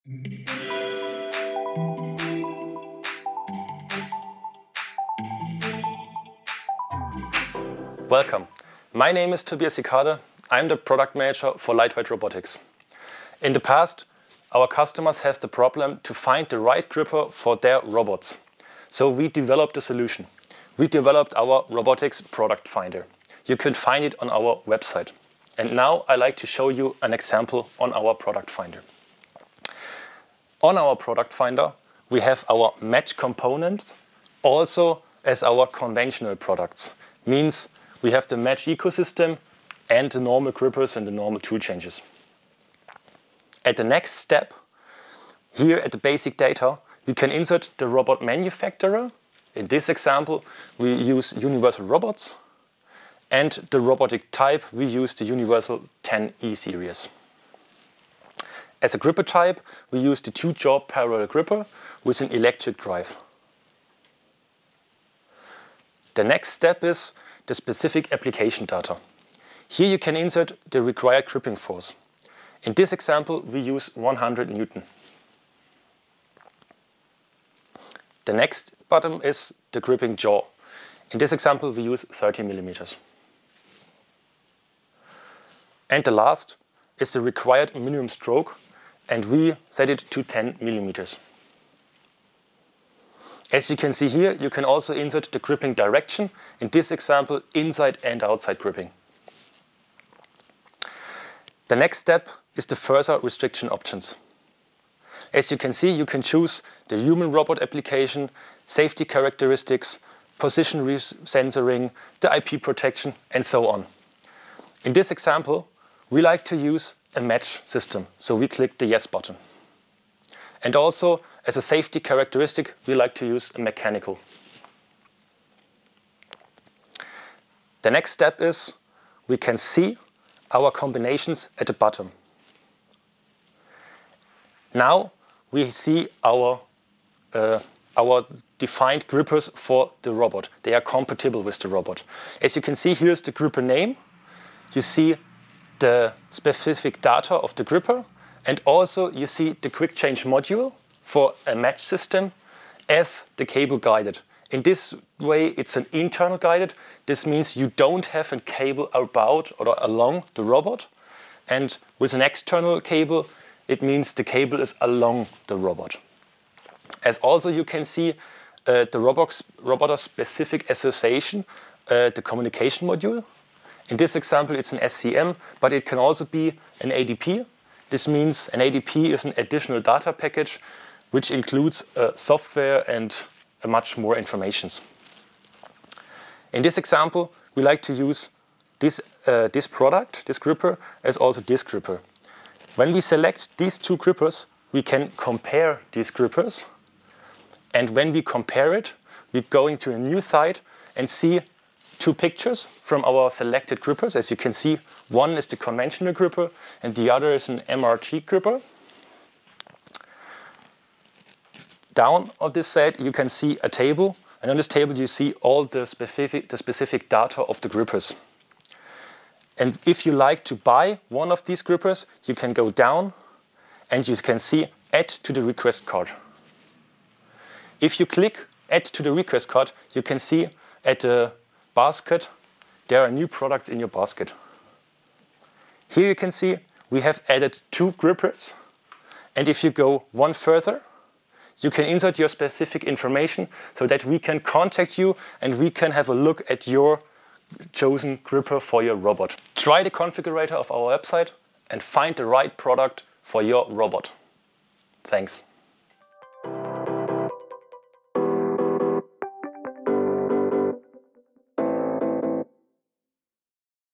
Find the right gripper for your robot quickly!
Our robotics product finder will help you to quickly find the right gripper for your existing robot. Just one click to the request shopping cart!
Learn more now in the video with our expert Tobias Hikade (Product Manager Lightweight Robotics) or at: